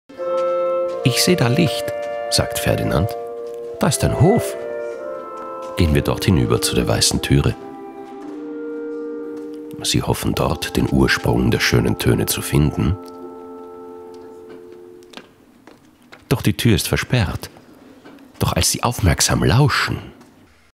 Was bisher geschah: Tag 7
Franziska & Ferdinand erkunden ein Haus, in dem wundersame weihnachtliche Musik zu hören ist...